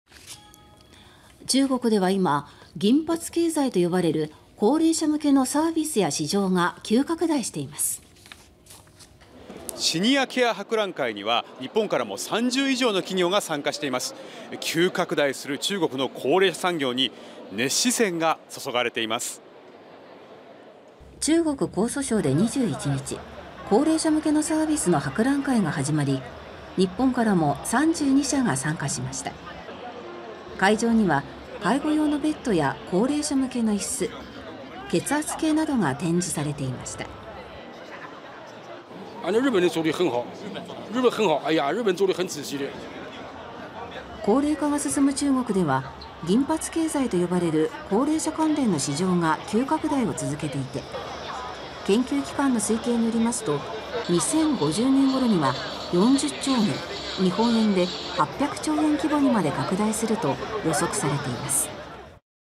中国で「銀髪経済」急拡大 高齢者向けサービスに熱視線(2023年9月23日)
中国では今、「銀髪経済」と呼ばれる高齢者向けのサービスや市場が急拡大しています。

　中国・江蘇省で21日、高齢者向けのサービスの博覧会が始まり、日本からも32社が参加しました。

　会場には、介護用のベッドや高齢者向けの椅子、血圧計などが展示されていました。

　来場者：「日本のサービスはとても良いよ。細かくて丁寧だ」

　高齢化が進む中国では「銀髪経済」と呼ばれる高齢者関連の市場が急拡大を続けていて、研究機関の推計によりますと、2050年ごろには40兆元＝日本円で800兆円規模にまで拡大すると予測されています。